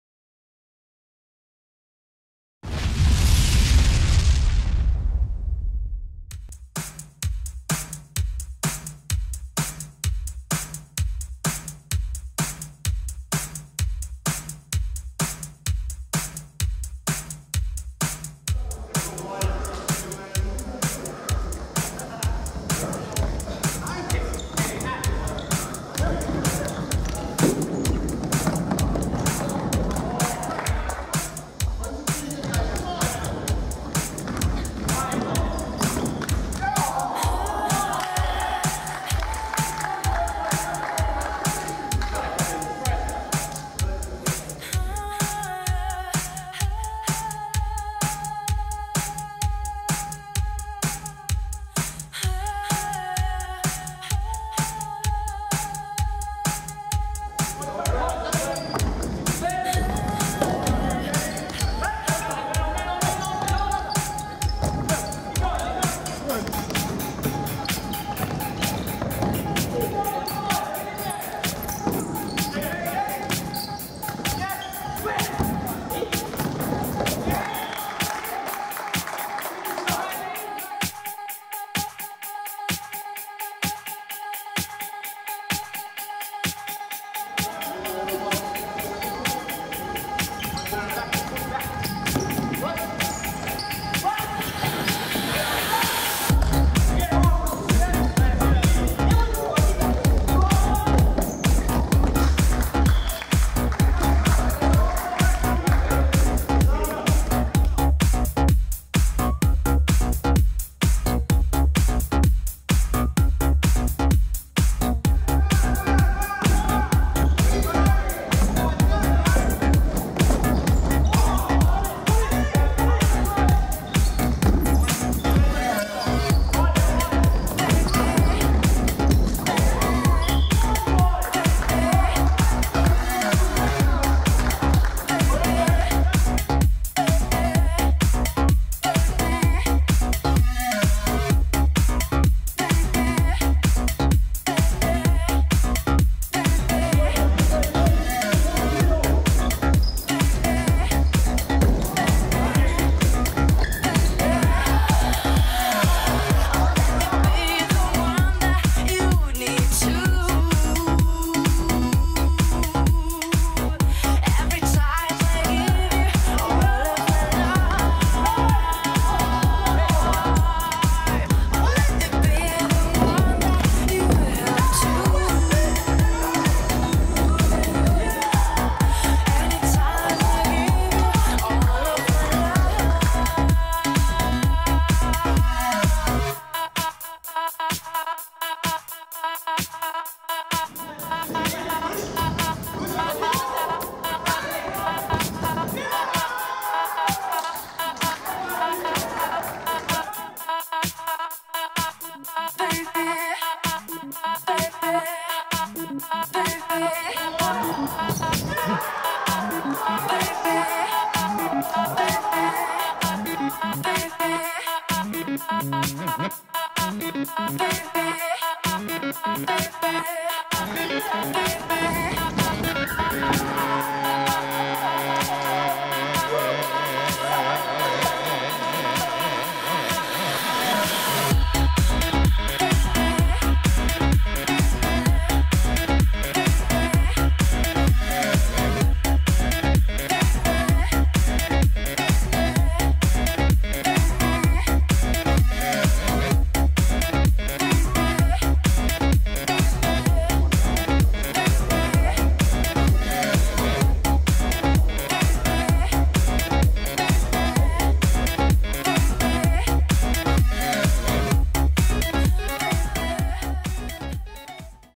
Futsal Super5s Hawthorn Men's Grand Final : Huracan v Midnight Marauders 15/3/2015
Futsal Super 5s - Hawthorn Men's League - Huracan v Midnight Marauders Sunday 15th March 2015.
Match Venue: Auburn High School Recreation Centre, Burgess Street, Hawthorn East - Highlights Video.